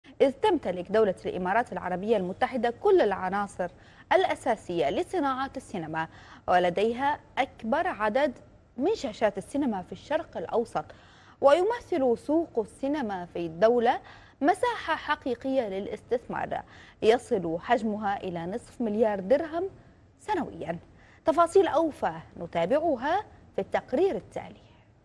السينما الإماراتية.. إنتعاشة حقيقية وقفزة ملحوظة في إنتاج الأفلام
شاهد آيضا:
قيادي بعثي يفضح تاريخ آل الأسد!؟
ملفنا الأخير والواسع من برنامج الخليج في أسبوع، عن السينما الإماراتية، إذ تمتلك دولة الإمارات كل العناصر لصناعة السينما، و لديها أكبر عدد من شاشات السينما في الشرق الأوسط، ويمثل سوق السينما في الدولة مساحة حقيقية للاستثمار، يصل حجمها إلى نصف مليار درهم سنويا.

استطاعت السينما الإماراتية أن تحقق انفراجة وانتعاشة حقيقية، أدت إلى حراك فني على أساسه تم إنتاج أفلام محلية، وضعت بصمتها في مهرجانات سينمائية مختلفة، وتصدرها قائمة صالات العرض.

كانت النغمة السائدة في الماضي، استهجان البعض وجود مهرجانات سينمائية في دول لا تصنع سينما، ومع توالي السنين، تبلورت حقيقة أنها كانت الحافز الأقوى لظهور عديد من الأفلام في المنطقة عموما، والإمارات خصوصا، حيث أسهمت المهرجانات من خلال برامجها المختلفة ودعمها للفيلم المحلي، في نقل خبرات وتجارب أخرى للسينمائيين بالدولة، ومنحتهم الثقة في إنتاجياتهم بعد عرض أفلامهم.

وفي السنوات الخمس الأخيرة، تكاتف بعض صناع السينما المحليين، لتقديم بعض الأفلام السينمائية الروائية الطويلة كتجارب مبدئية، وظلت السينما الإماراتية تتحرك إلى الأمام بثقة كبيرة، حتى شهدت في العاميين السابقين قفزة ملحوظة في إنتاج عدد من الأفلام التي حققت نجاحا وصدى كبيرين في المهرجانات المحلية والعالمية ، ومن ثم عرضها تجاريا في صالات عرض السينما الإماراتية.

الإقبال الجماهير على مشاهدة الأفلام الإماراتية في صالات العرض، والدعم المتوافر من بعض الجهات المعنية بالفن في الإمارات .. أمر رفع عدد الإماراتيين الشباب المهتمين بصناع السينما والفن السابع، ورغبة أغلبهم في الاتجاه نحو تنفيذ أفلام روائية طويلة تكاد تكون منافسا قويا للأفلام العربية والعالمية، فقد أنتجوها بأعلى جودة وبأفضل التقنيات، الأمر الذي جعل السينما الإماراتية تسير في تألق في فضاء الفن السابع.

بعض الإنجازات السينمائية التي تحققت في السنوات الماضية، كان الفيلم الإماراتي الكوميدي "ضحى في أبوظبي"، عرض الفيلم في مارس الماضي، ولامس الواقع الإجتماعي، وأثبت أن الفيلم الإماراتي قادر على المنافسة في شباك التذاكر.

واليوم يطل علينا فيلم جديد يجرى تصويره في العاصمة أبوظبي بمشاركة عدد من النجوم الإماراتيين، تحت عنوان ضاعن وفلونة في الإمبراطور، على أن يعرض خلال العام الجاري.